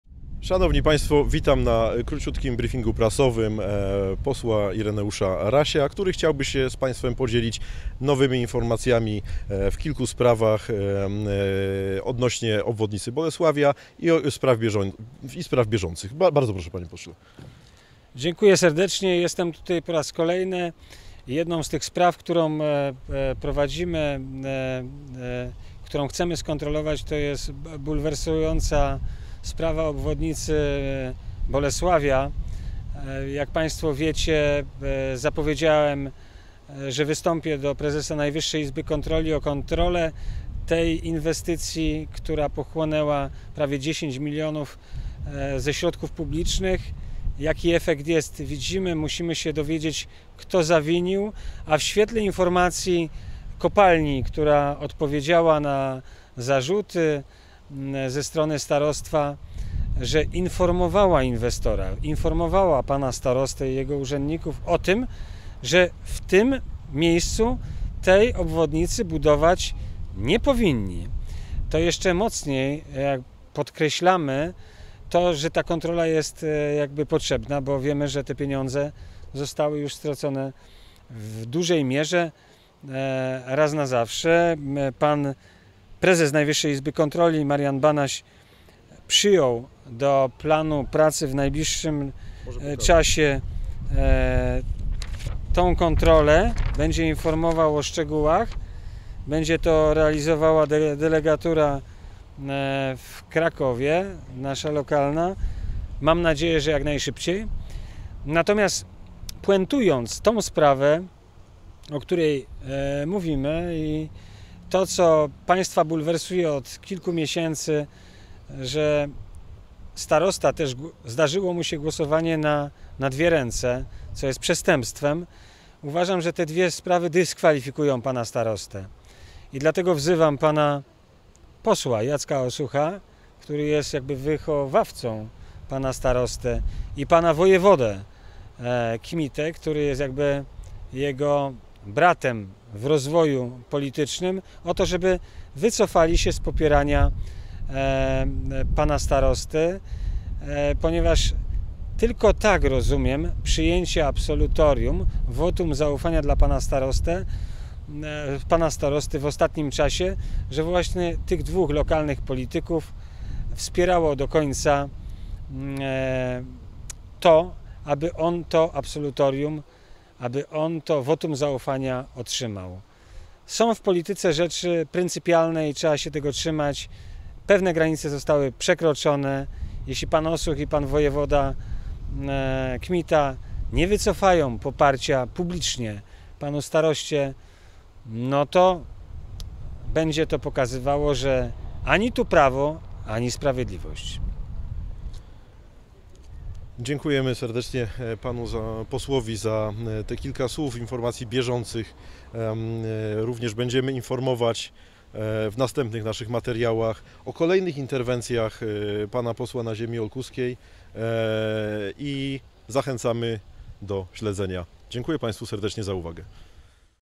Marian Banaś zadecydował: będzie kontrola NIK w Starostwie Powiatowym w Olkuszu!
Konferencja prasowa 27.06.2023
Ireneusz Raś, Miłosz Pałka